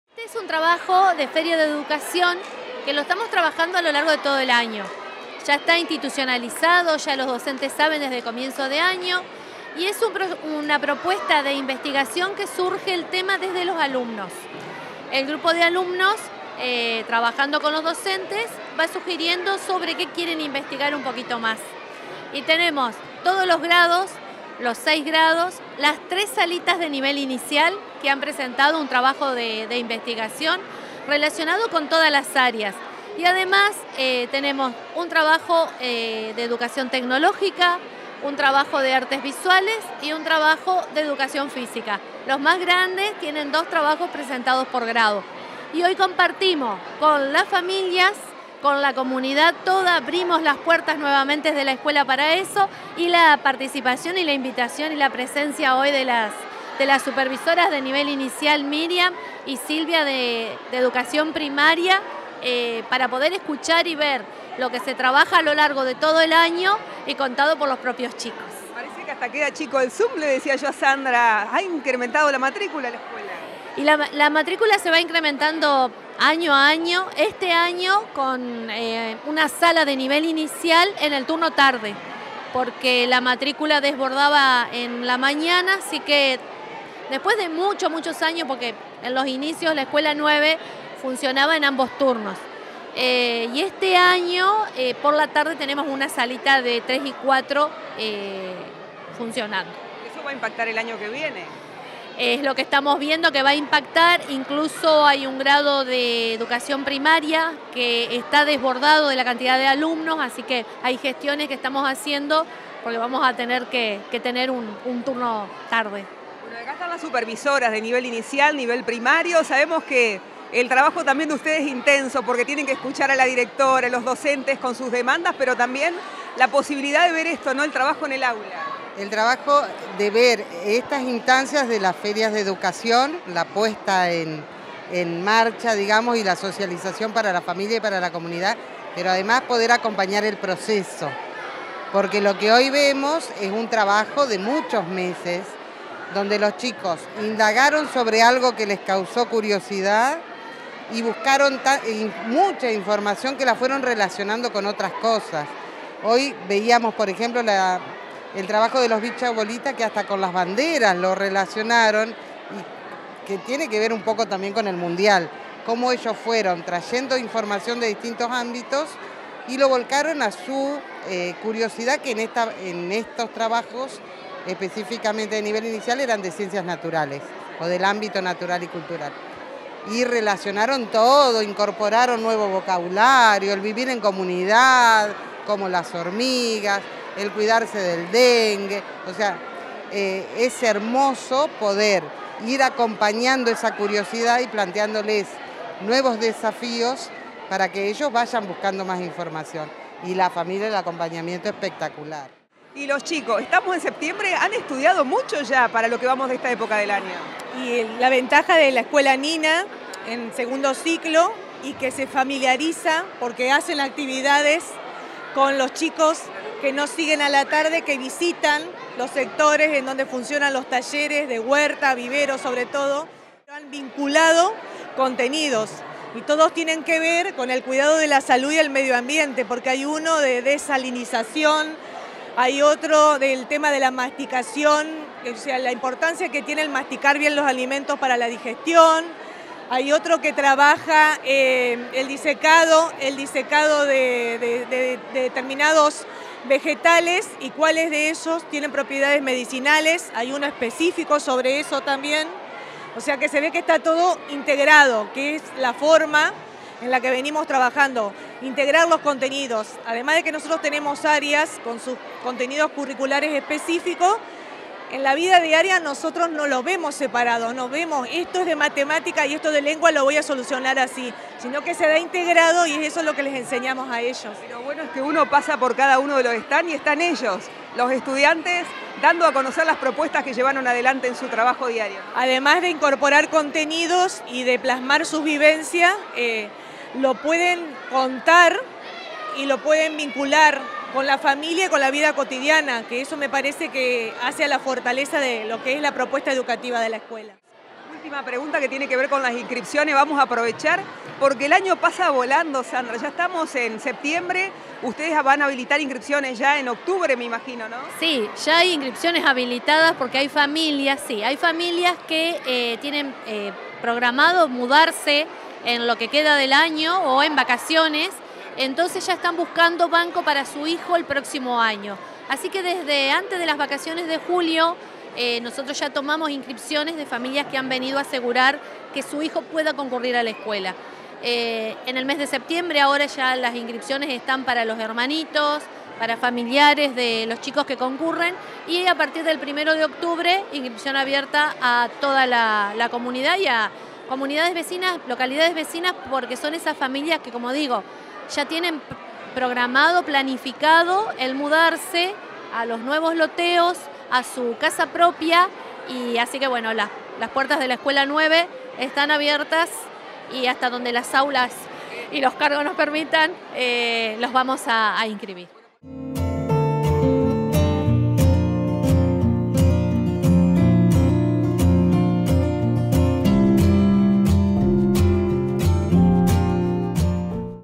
ALDEA BRASILERA - ENTRE RÍOS
🔖📚Se realizó la Feria de educación con la presentación de diversos proyectos vinculados a la salud, medioambiente y comunidad, en la Escuela N°9 "Brigadier General Justo José de Urquiza".